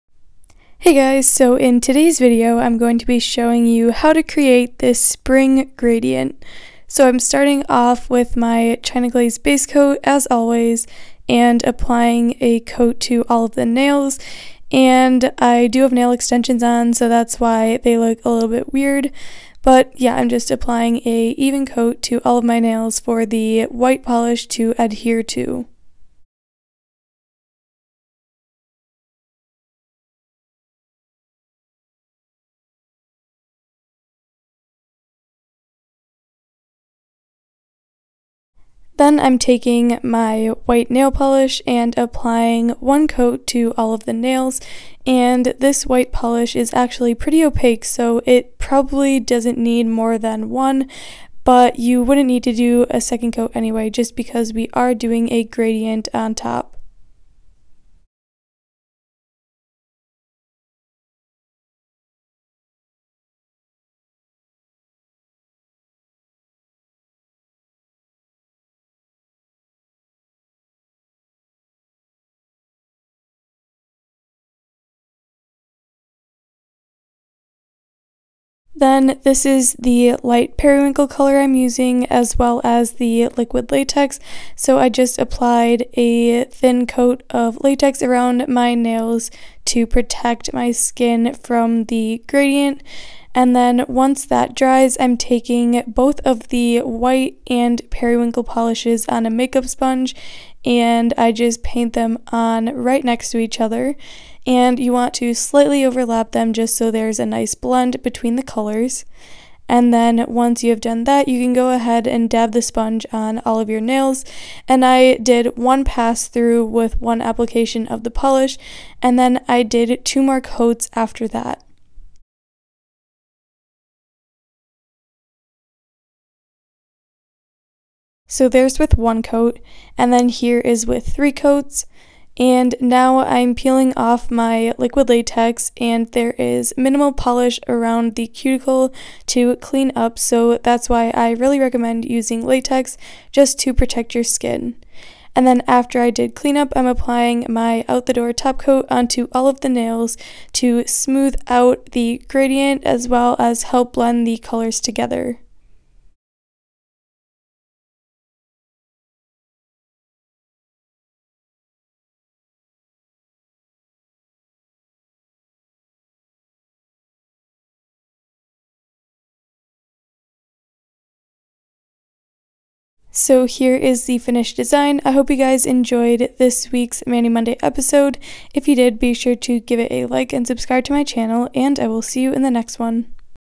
MANI MONDAY | spring gradient
hey guys! in today’s mani monday episode i’ll be sharing how to create this super springy and pastel gradient. enjoy!